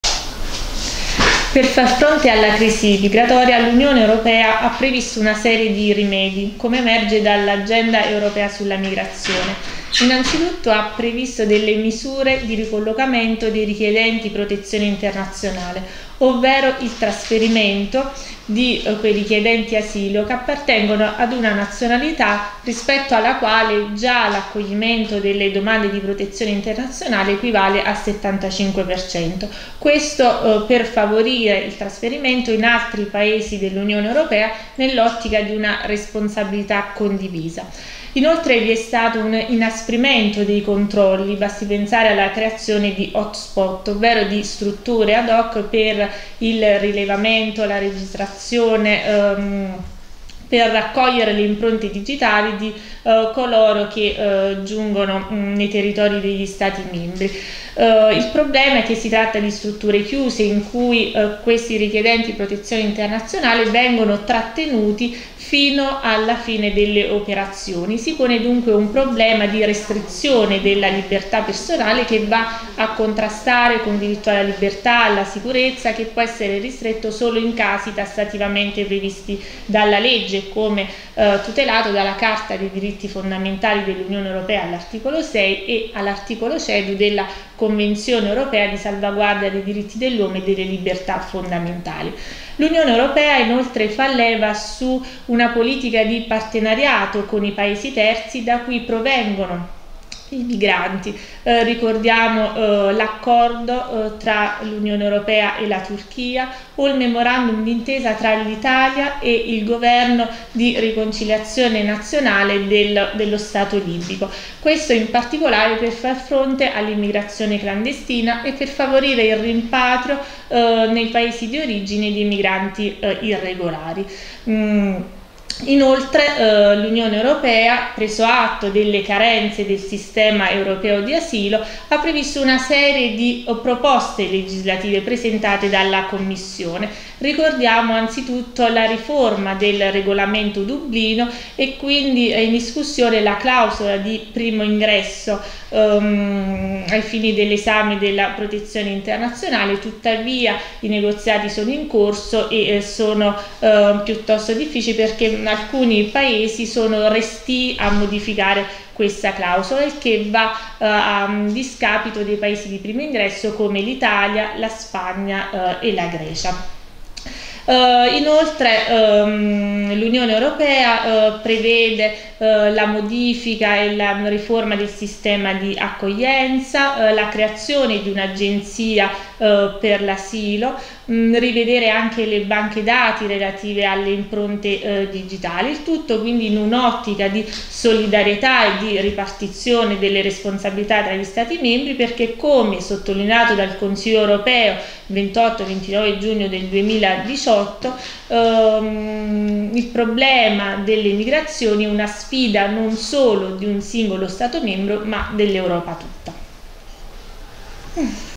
Mödulo 4.1.2 Anna Iermano Universidad de Salerno Normativa europea en asilo y refugio
por Unidad de Innovación